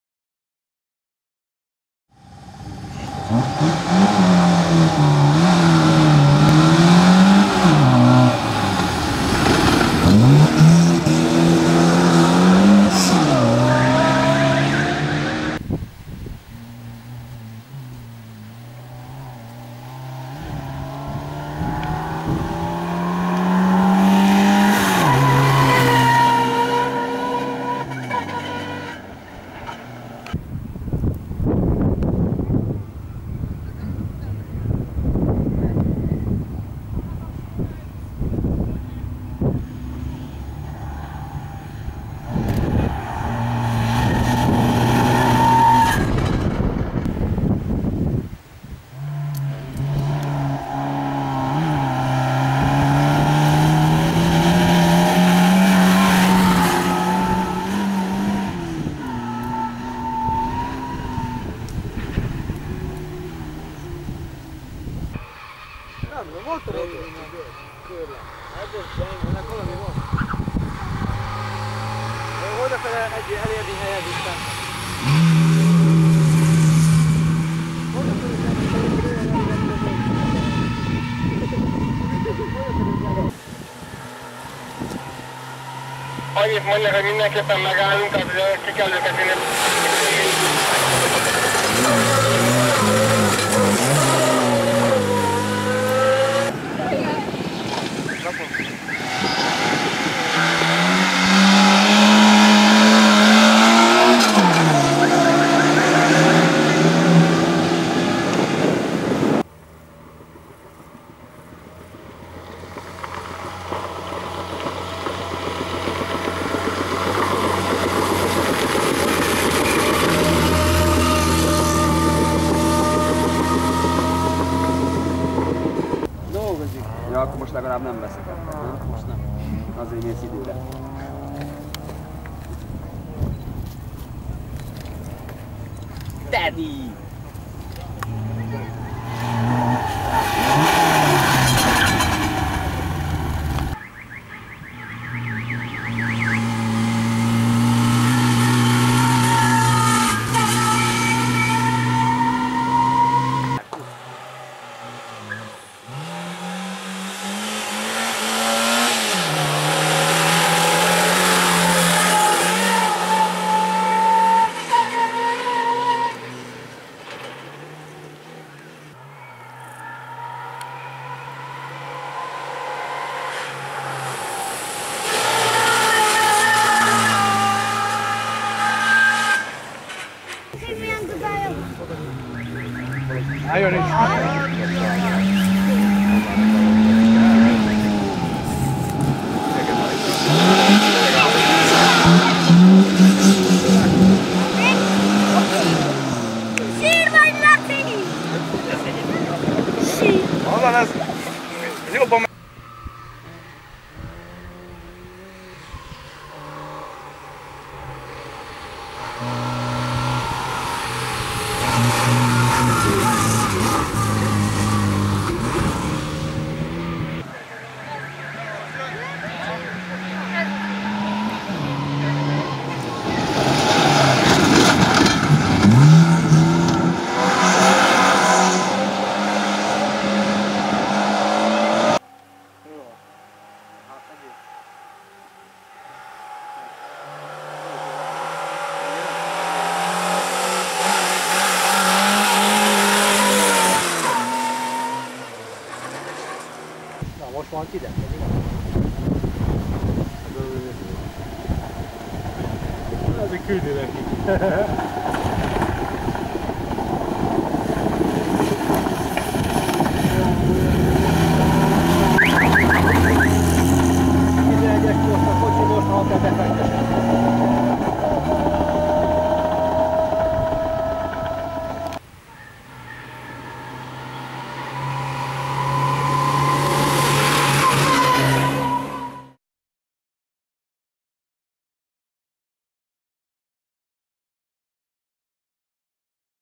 Tóth László-Lada 2101 Nagynyárádi Autós Show 2016.-Lepold Sportvideo
Tóth László versenyzői filmje,a 2016-os Nagynyárádi minirallye-ról.Vágó:Dénes Szabolcs Szerkesztő:Lepold Zsolt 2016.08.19.